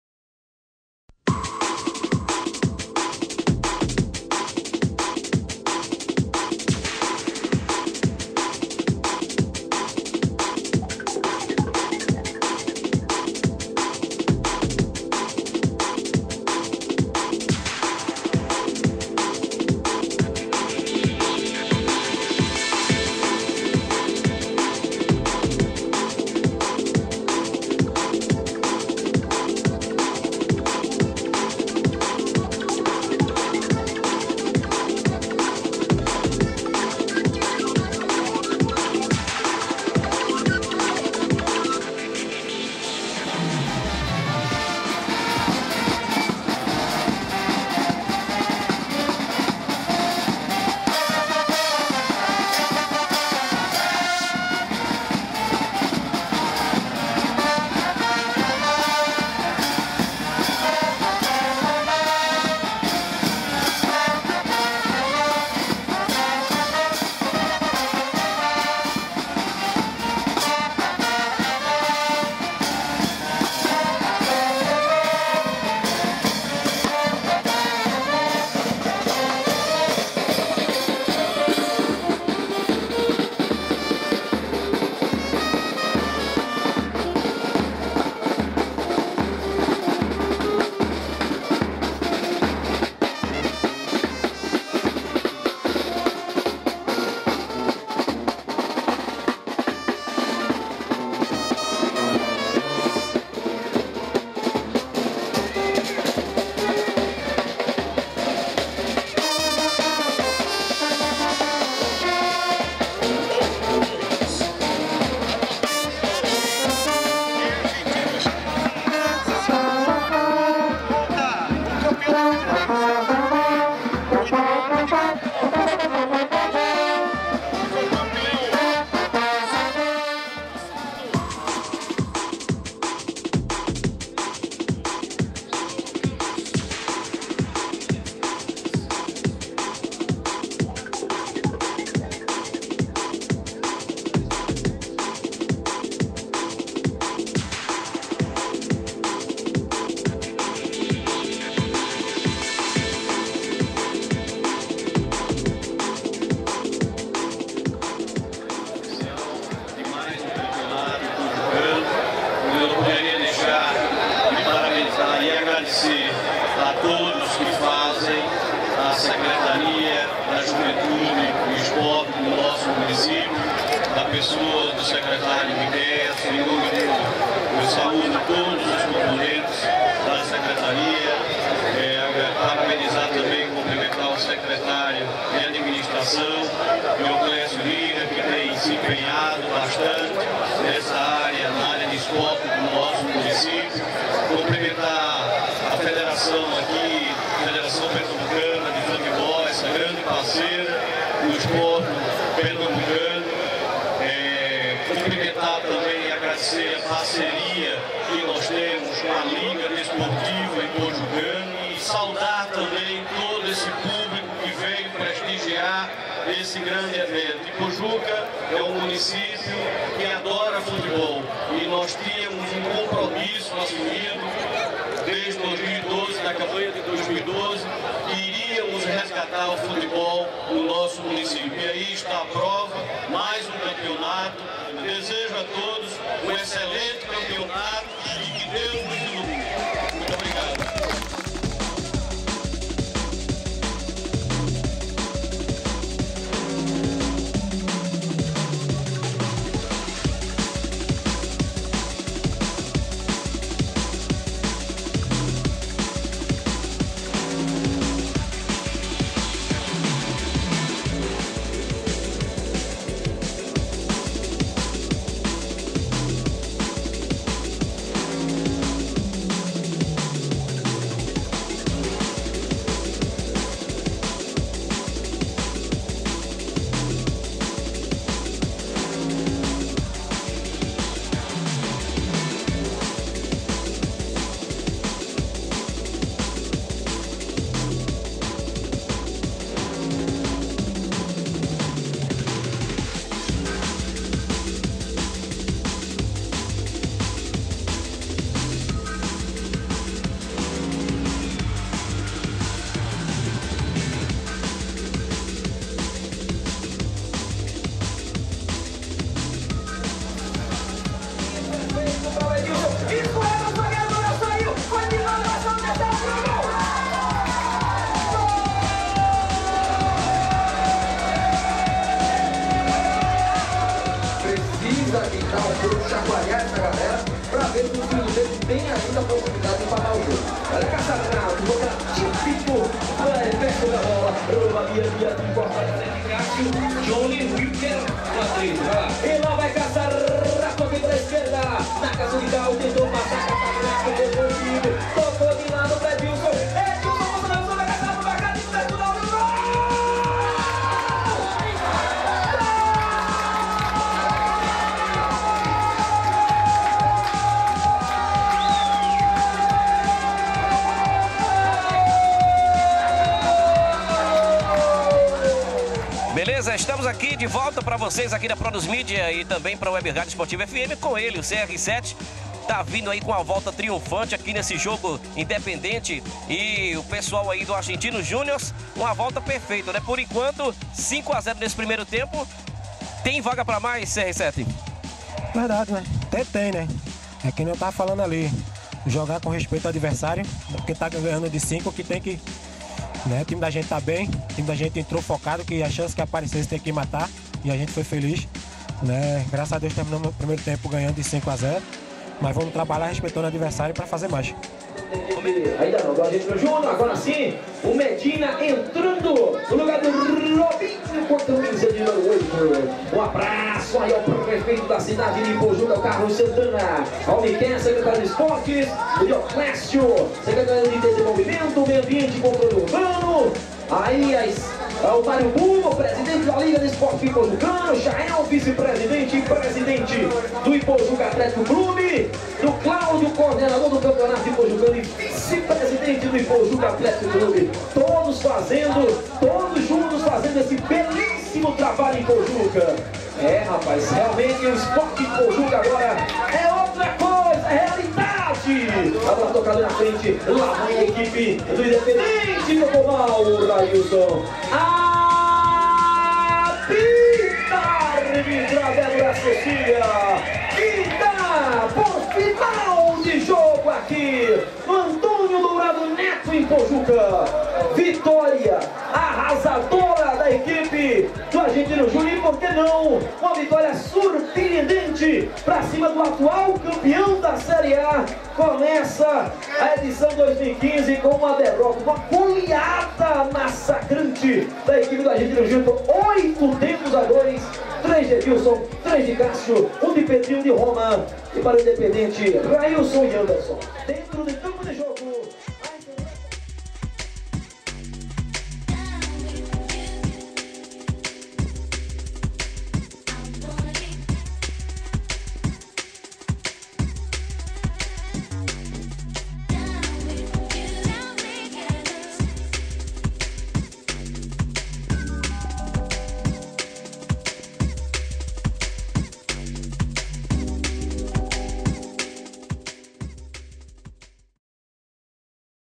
ABERTURA DO XV CAMPEONATO IPOJUCANO 2015
ABERTURA DO XV CAMPEONATO IPOJUCANO 20115, REALIZADO NO ESTADIO Dr. ANTONIO DOURADO NETO, NO DIA 14/10/2015, COM O JOGO DE ABERTURA ENTRE O FORTE ARGENTINO Jr DO CAÇA-RATO CONTRA O CAMPEÃO DA 1ª DIVISÃO DE 2014 O INDEPENDENTE.